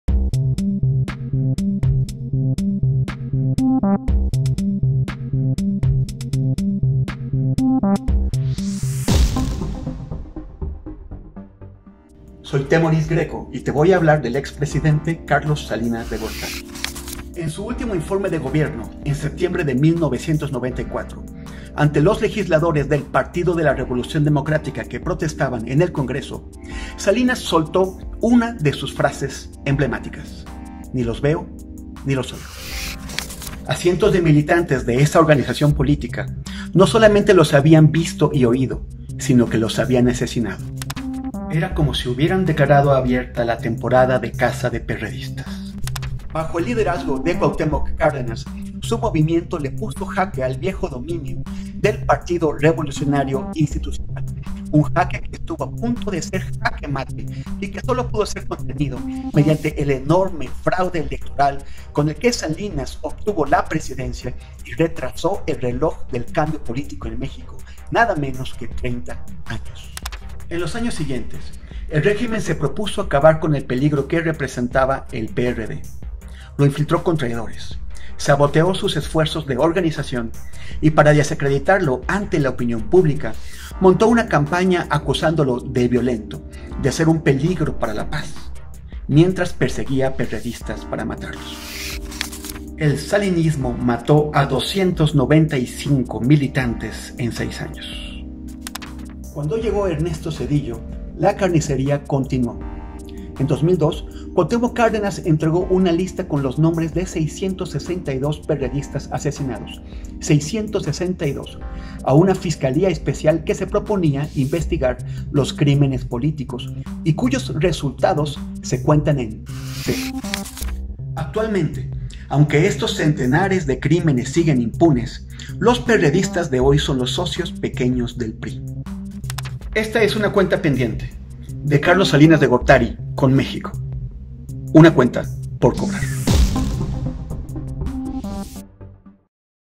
Cuentas por cobrar, el poder de confrontar al poder | Carlos Salinas de Gortari
En esta serie de cápsulas Témoris Grecko aborda distintos temas que causaron controversias durante los sexenios de los expresidentes mexicanos.